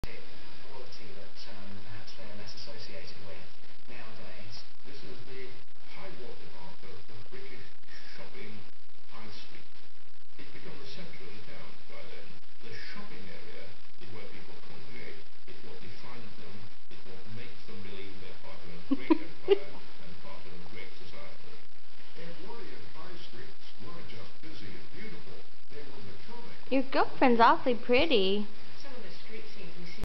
Re: DOG ROMANCE
This is Cosmo as a pup, loving on his girlfriend.  She looks suspiciously like your dog's girlfriend.  You should see girlfriend now - no coat, no pink bows, missing some stuffing and very, VERY dirty.